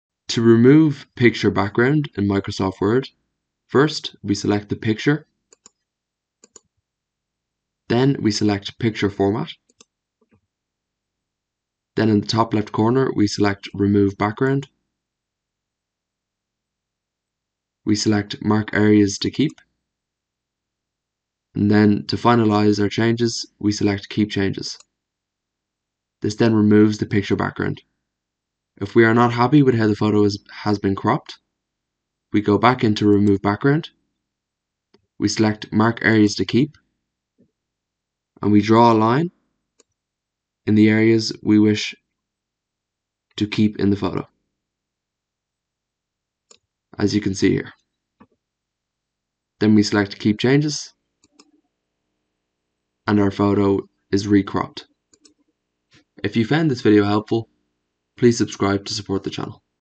How to Remove Picture Background in Microsoft Word in 2023
This video shows you how to remove the background from an Image in a Microsoft Word doc. You need to select the 'picture format' and then choose the remove background option.